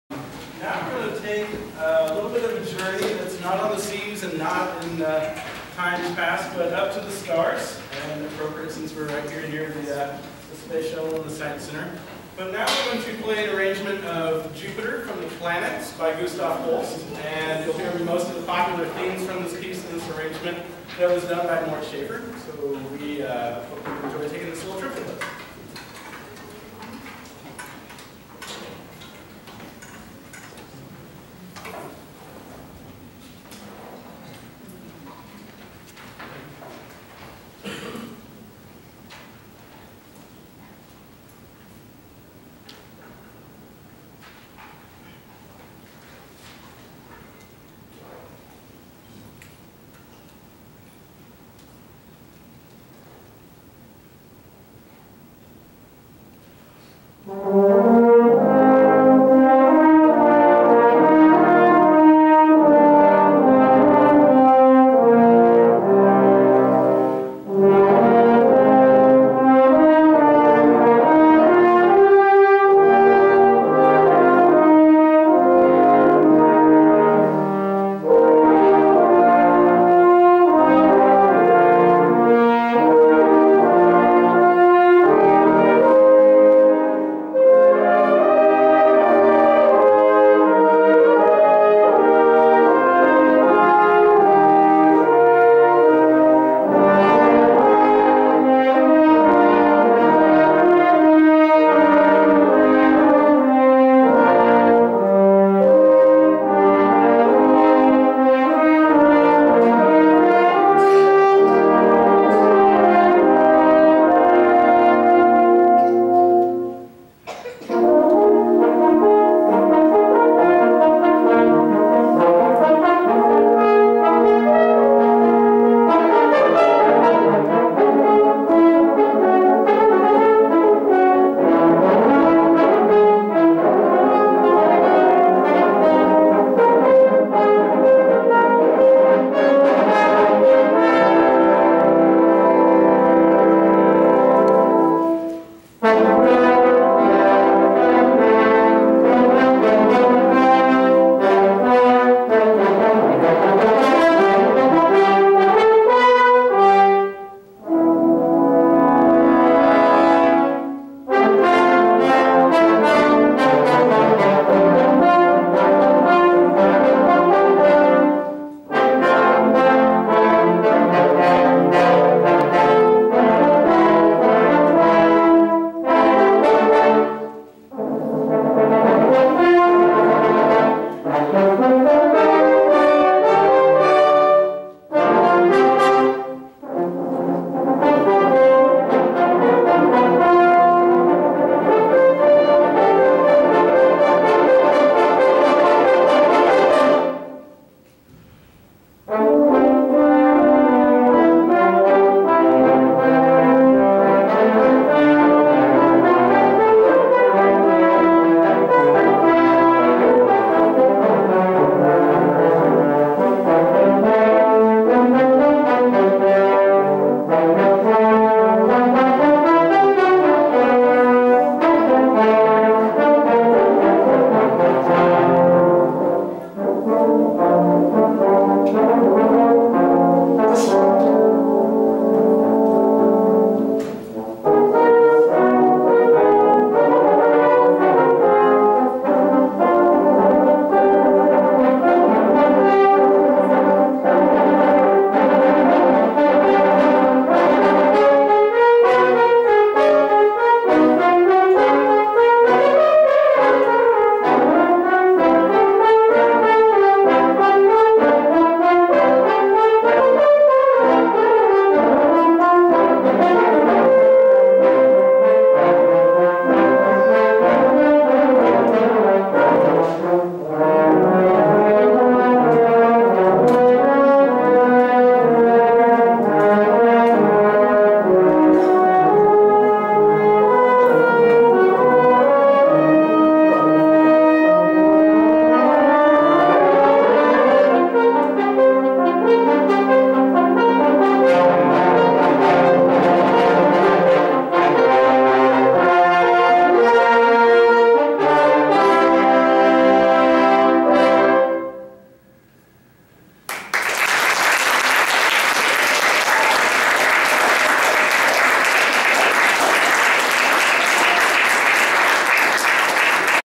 USC Horn Choir Christmas YOLA Performance 2014 –3
This is the University of Southern California Horn Studio's students performing at the Christmas YOLA performance. Enjoy the wonderful horn music.